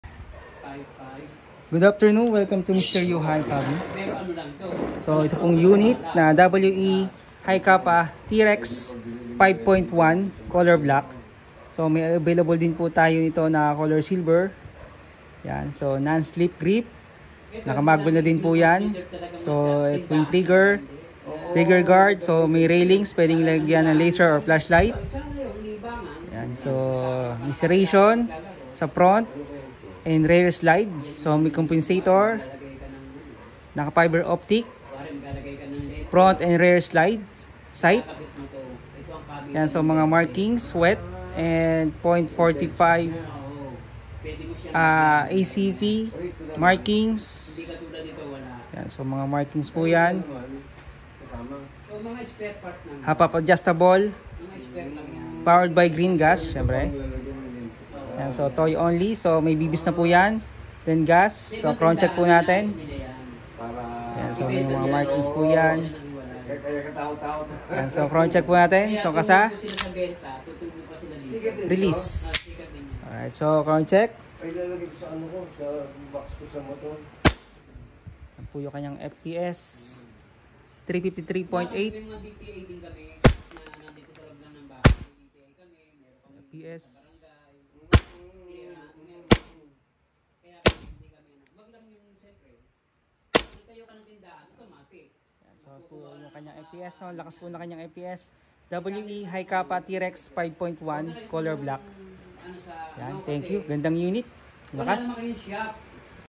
Review and Chrono Test WE TREX HI CAPA 5.1 COLOR BLACK GBB PISTOL
ToyOnly 🔫